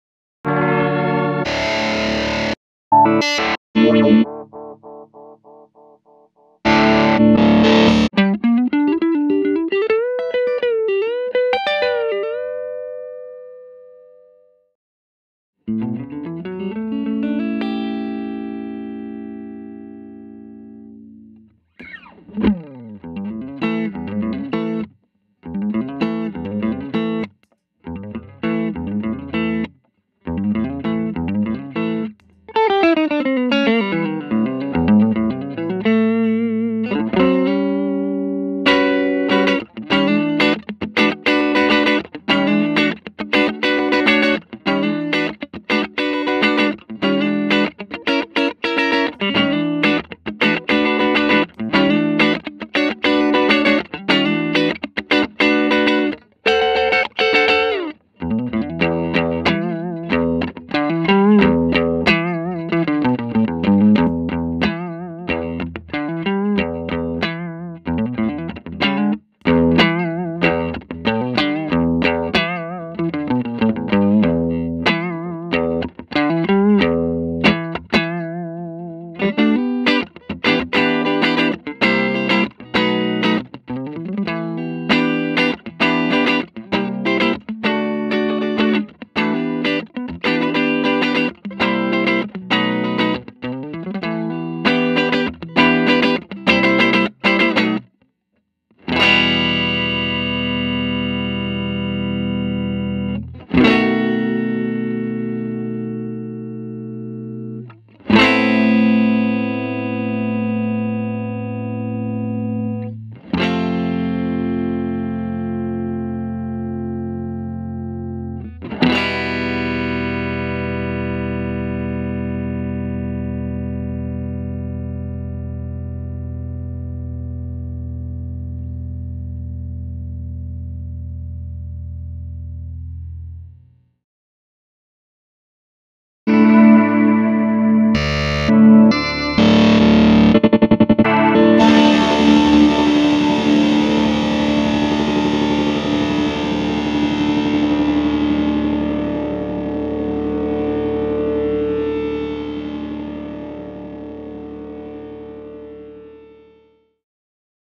EarthQuaker Devices The Warden Optical Compressor
EarthQuaker Devices The Warden Optical Compressor. Video & animation by Jeff France, audio recording by Ben Vehorn, guitar by Cory Juba. Gear used: EarthQuaker Devices Tele w/ lollar pickups, The Warden, DR. Z KT45, Voodoo Labs ISO 5, Lava Cable. for more info.